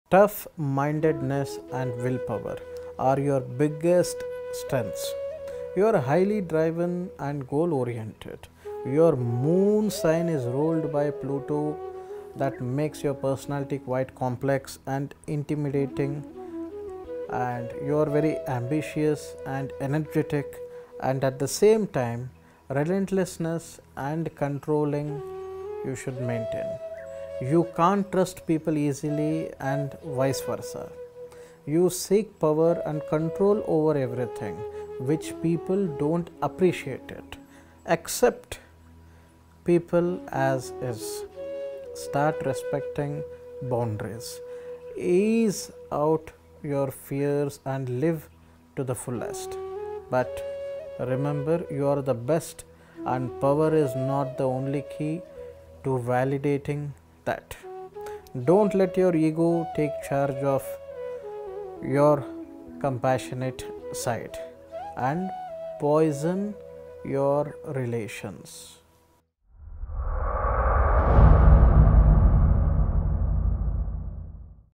Characteristics of Scorpio....
Scorpio is ruled by Pluto, the planet that governs both destruction and transformation. On a good day, Scorpion energy is ambitious and enticing. On a bad day, however, the shadowy side of Scorpio is fueled by a relentless desire for control. Power-hungry Scorpios must remember that if controlled by their egos, they are at risk of poisoning themselves. This sign is at its best when that intrinsic intensity is applied to deep, soulful connections with friends and lovers. When they build trust with others, Scorpios demonstrate unparalleled empathy, depth, and commitment that brighten even the darkest parts of Scorpio’s magical psyche. Watch the above video, where Pandit Jagannath Guruji explains everything about the Manipulative yet Passionate SCORPIO.